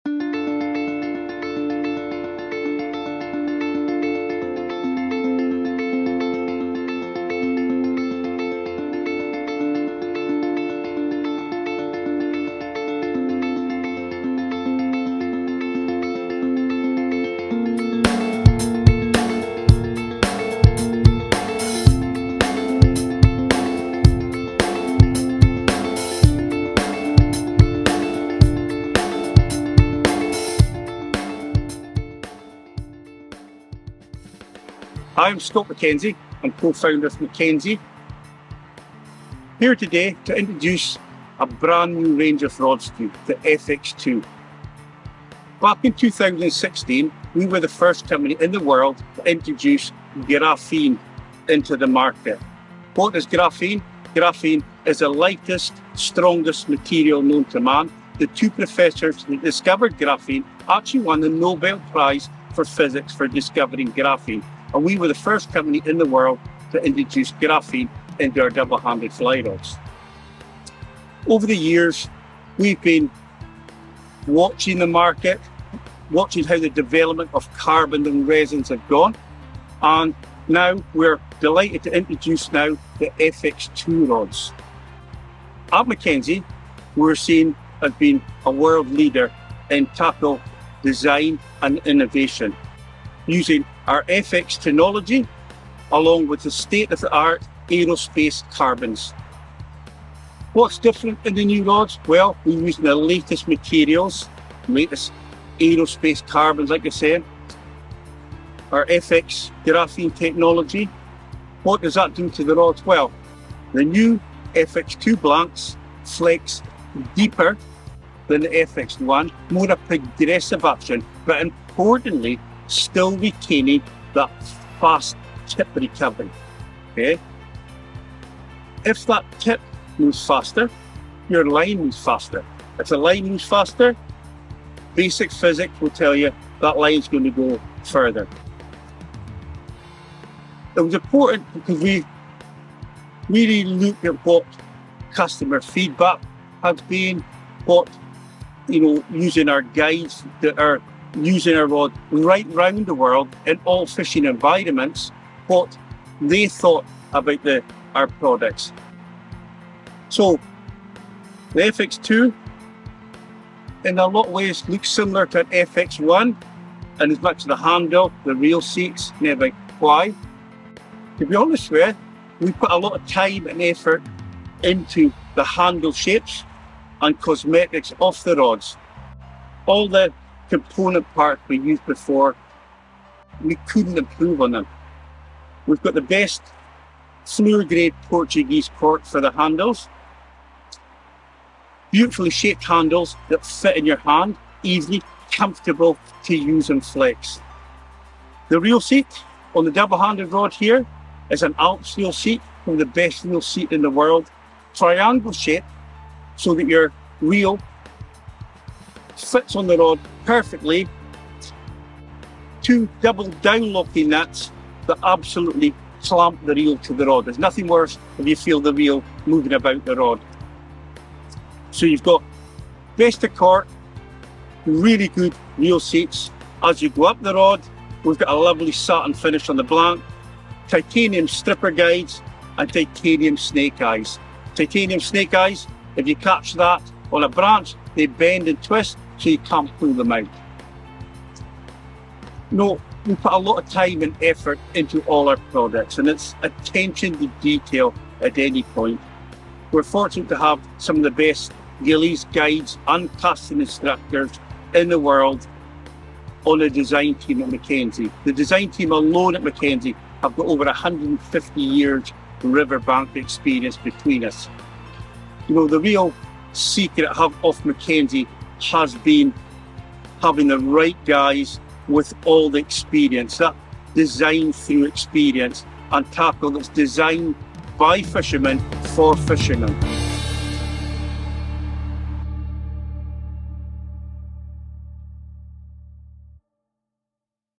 Mackenzie FX2 Fly Rods Trailer
FX2 use Graphene and a carbon normally only available to aerospace technologies to create our new range of rods that are amongst the lightest and strongest rods on the market today.

We have been true to our brand philosophies of designed by fishermen for fisherman and designed through experience. The FX2 range of rods, including single-handers, switch, double handed and the FX2 Atlas double-handed travel rods, all have a fairly deep and progressive action matched to fast tip recovery. These rods have reserves of power and yet are sensitive, making them truly user-friendly.

The light slate blue color blanks have contrasting whippings featuring titanium snake eyes and stripper guides. The rods come in a cloth bag and a beautiful orange rod tube with anodised black fittings.